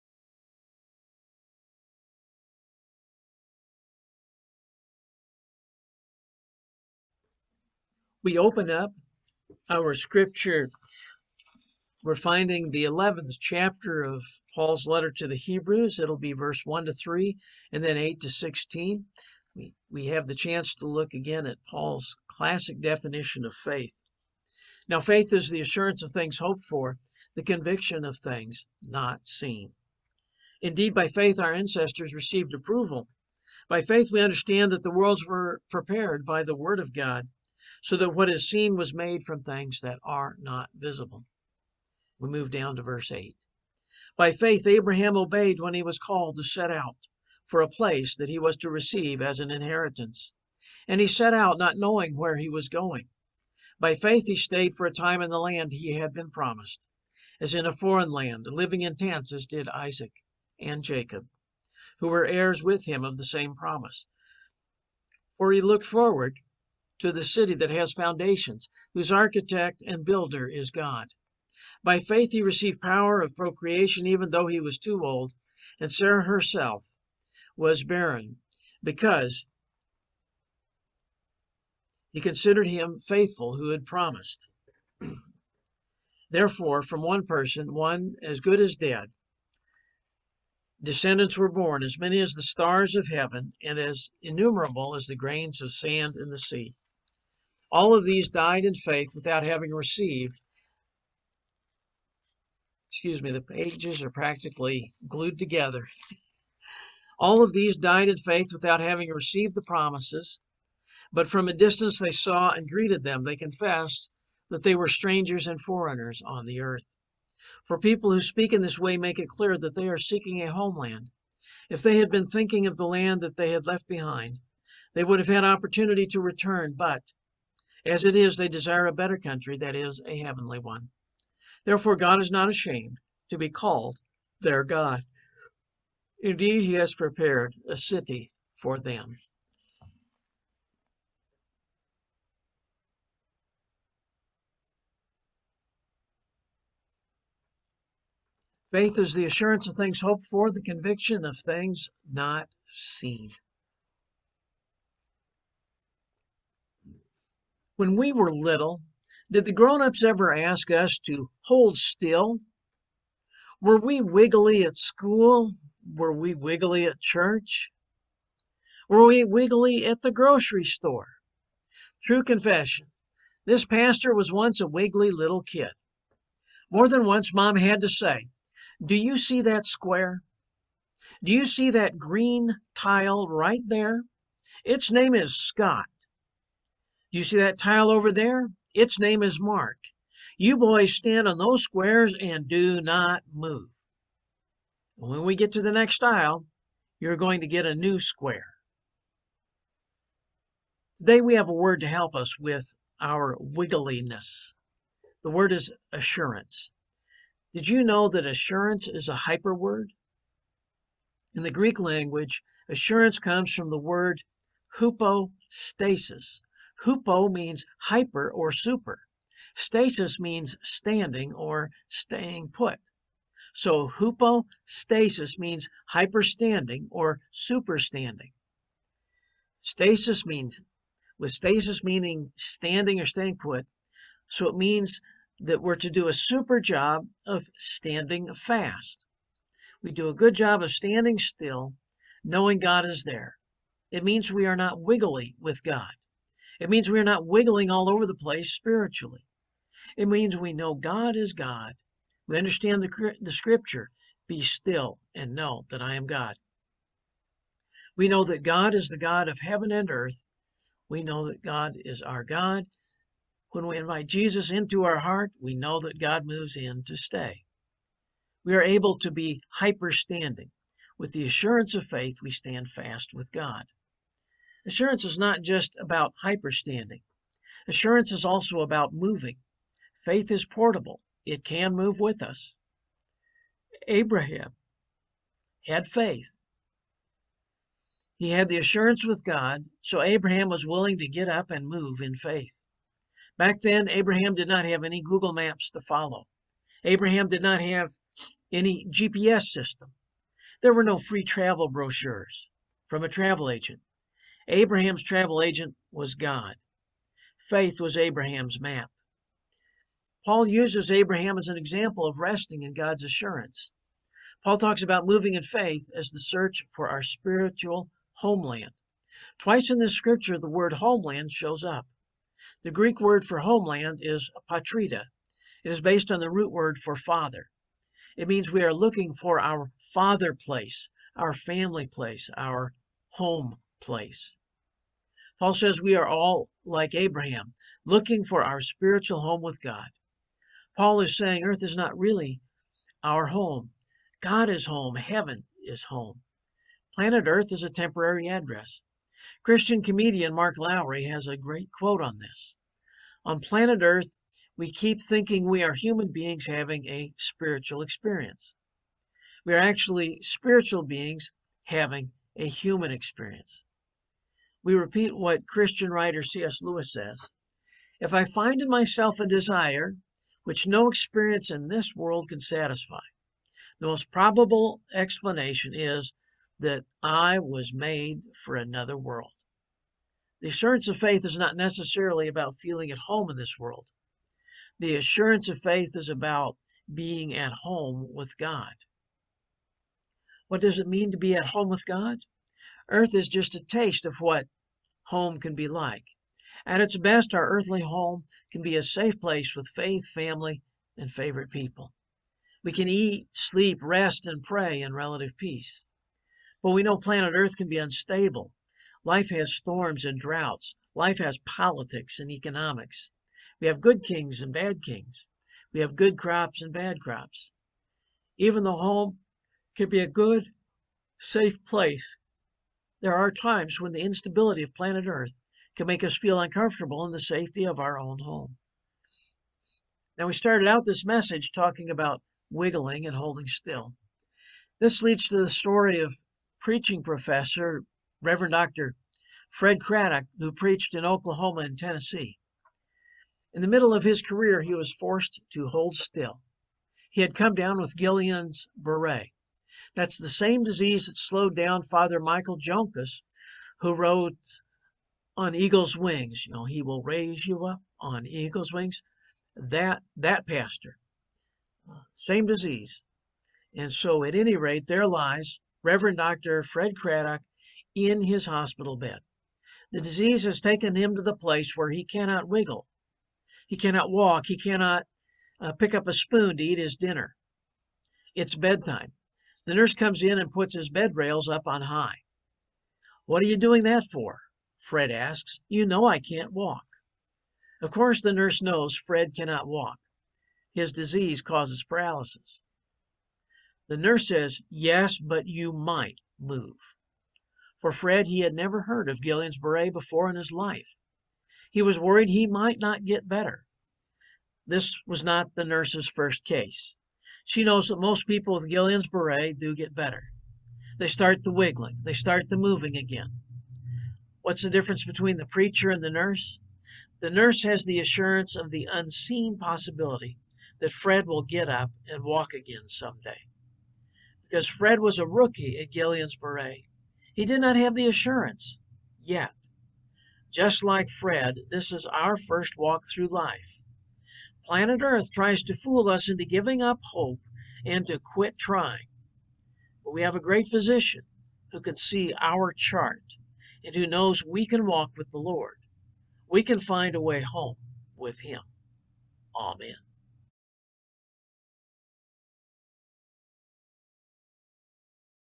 "The Assurance of Faith" 8:00 AM, August 7, 2022, Hebrews 11:1-3, 8-16, Doniphan-Rosedale UMC Parish
"The Assurance of Faith" 8:00 AM, August 7, 2022, Hebrews 11:1-3, 8-16,
Doniphan-Rosedale UMC Parish, Pastor Scott Evans: Paul delivers and explains his classic definition of faith.